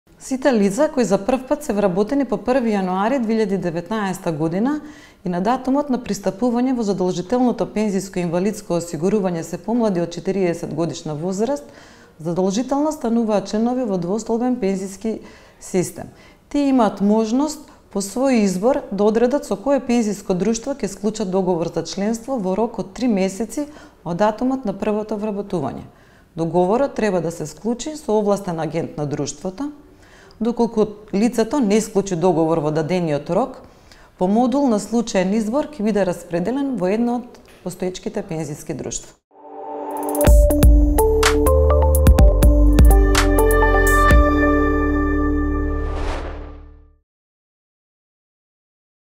Како да станам член во вториот пензиски столб?
Супервизор на капитално финансирано пензиско осигурување е Агенцијата за супервизија на капитално финансирано пензиско осигурување (МАПАС) тел.

Одговара Елизабета Коцев од КБ Прво Пензиско Друштво.